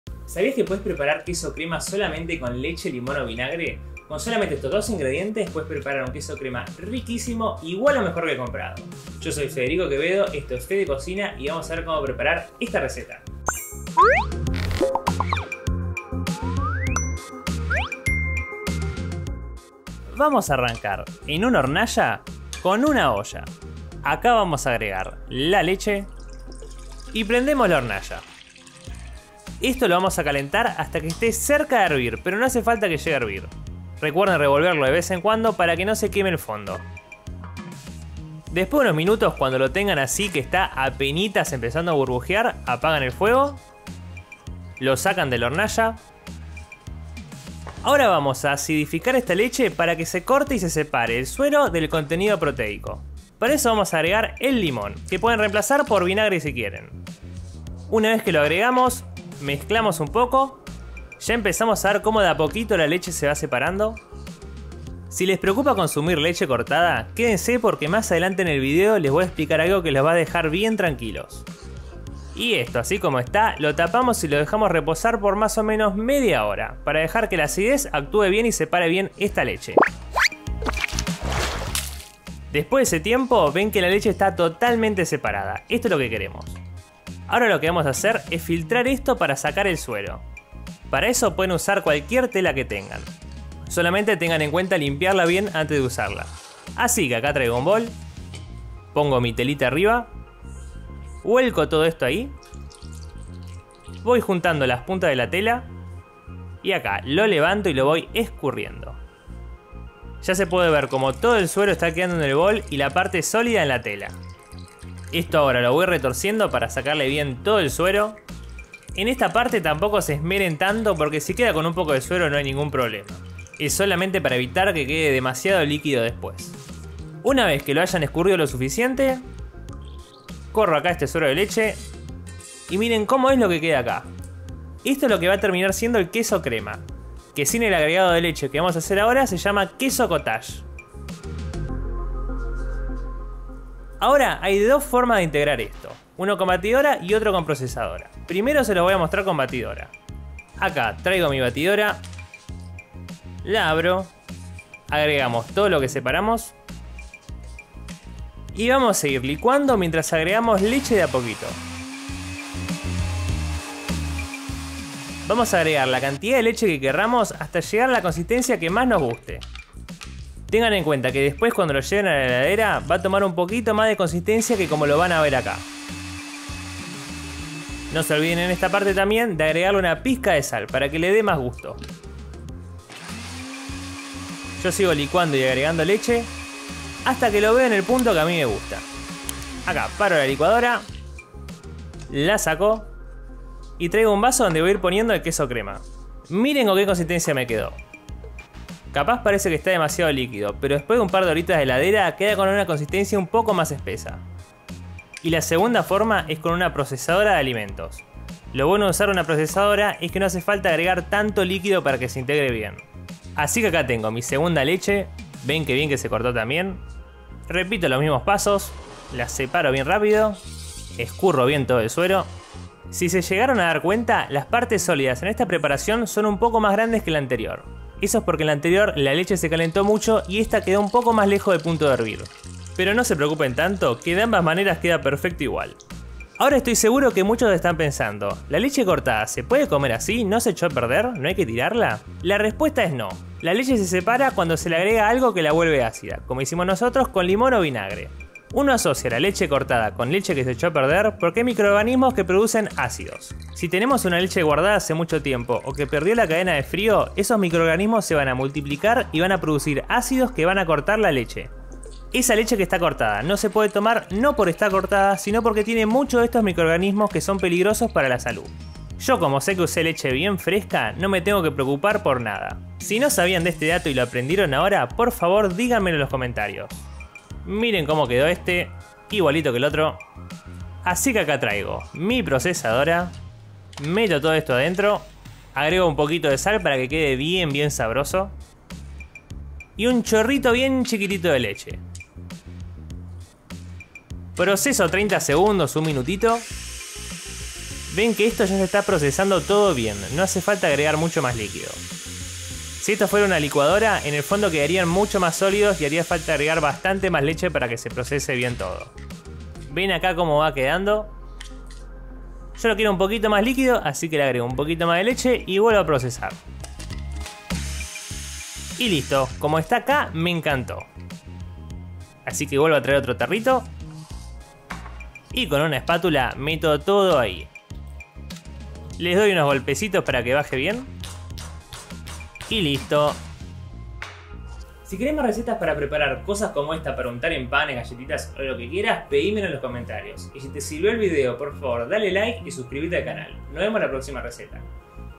Como Preparar Queso Crema
Aprende Como Preparar Queso Crema, bien facil y rapido y que ademas te va a quedar igual o mejor que el comprado. Para que nunca mas falte queso crema en tu heladera!

Queso crema
-1L leche
-60ml Limon (o Vinagre)